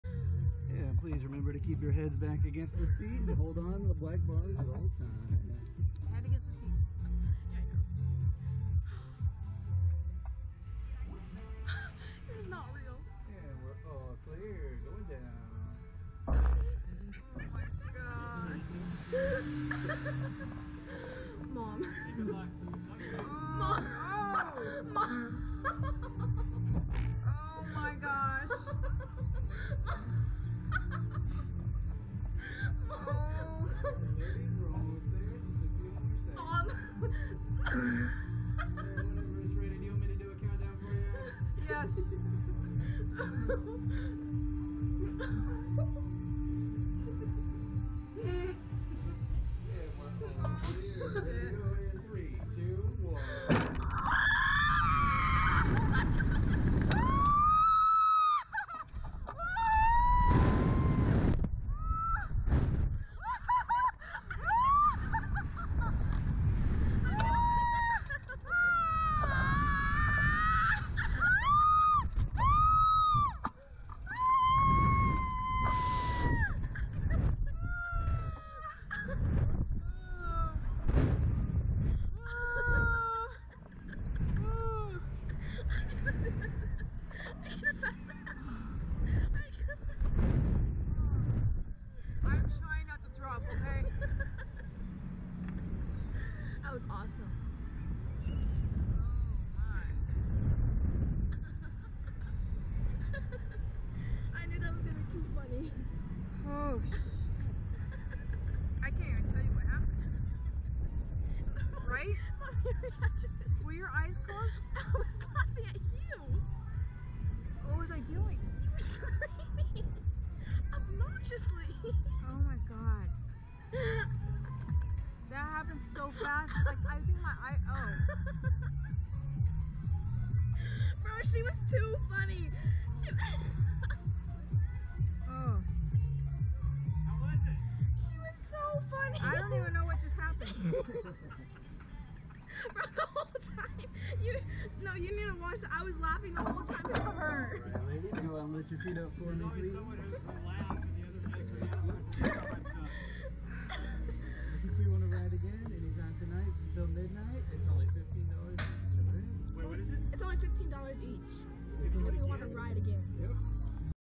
Steph & Kailie
400' tall attraction that launches riders at 100 m.p.h.